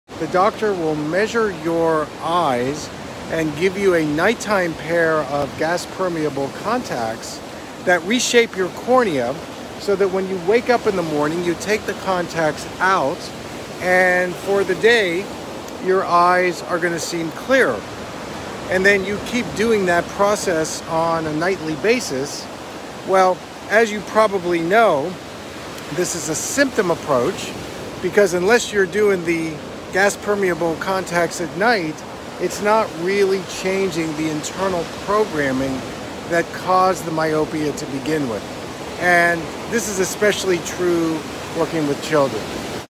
Symptom Based Approach
A downfall of a symptom-based approach to healing myopia.